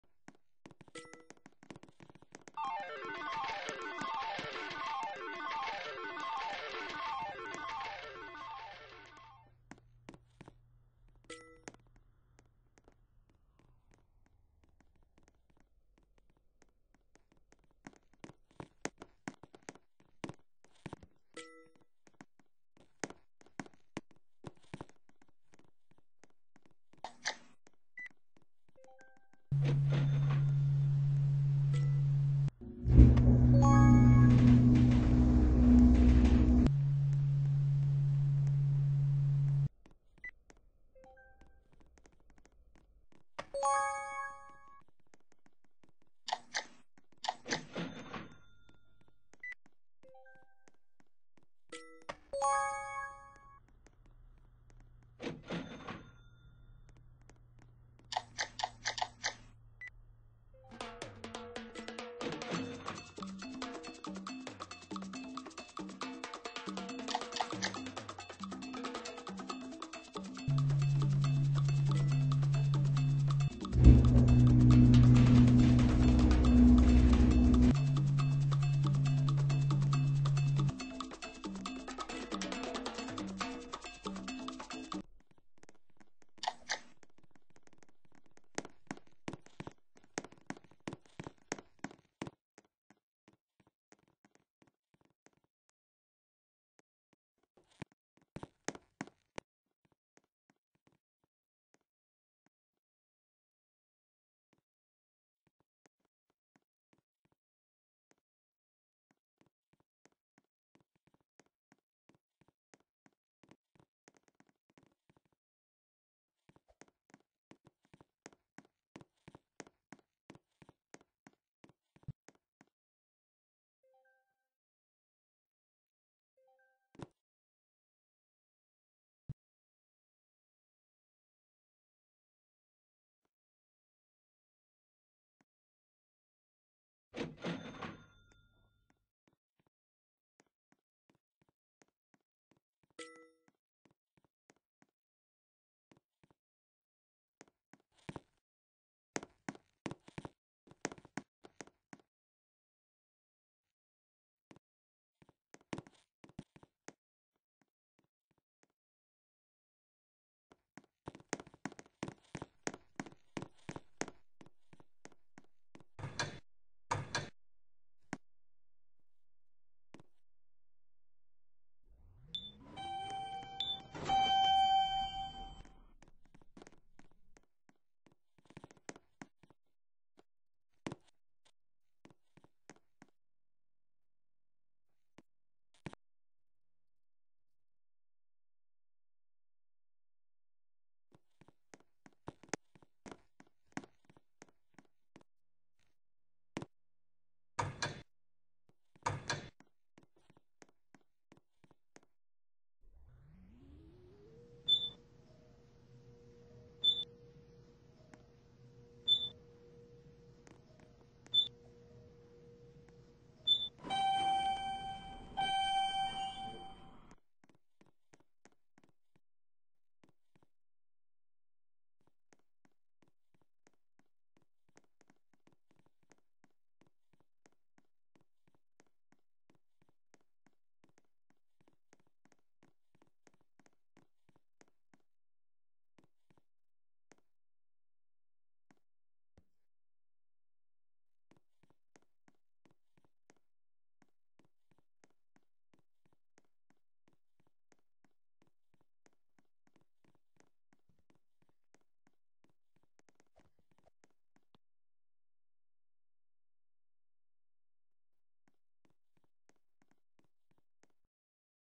Slap on mod: Illinois hydraulic elevator at Illinois Company HQ.
Information: This elevator used to be a dover with black, then a epco, now the innovation bruiser. The Motor pitch is a B and it still has its original motor.